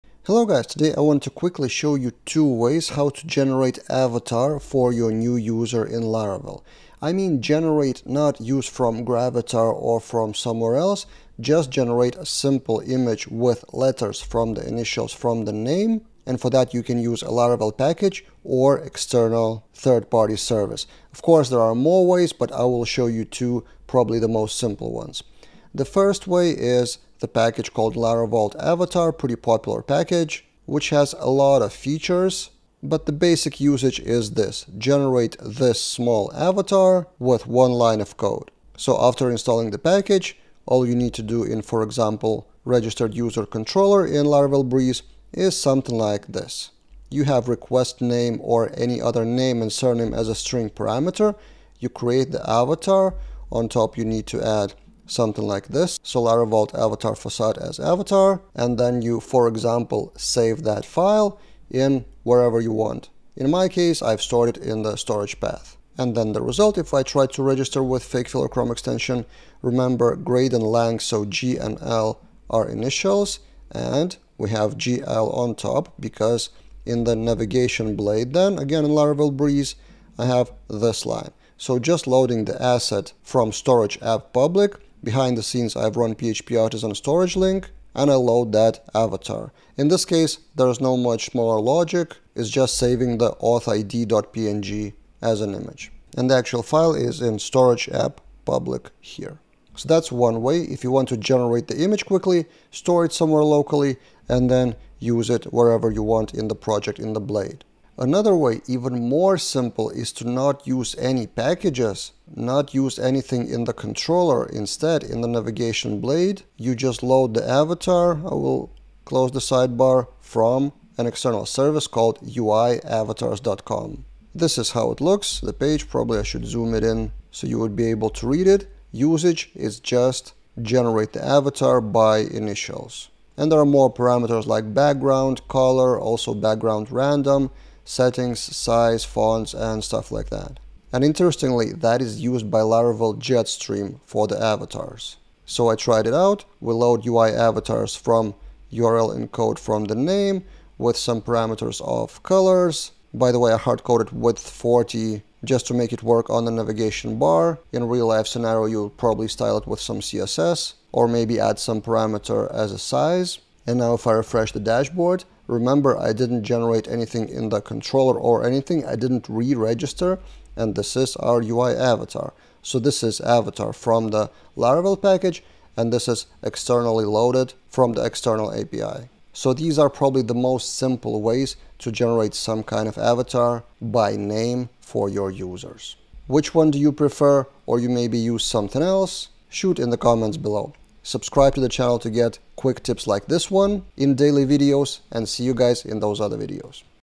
2 Simple Ways to Generate User Avatar in Laravel
I want to show you two different tools to generate the avatar image from the user's name/initials.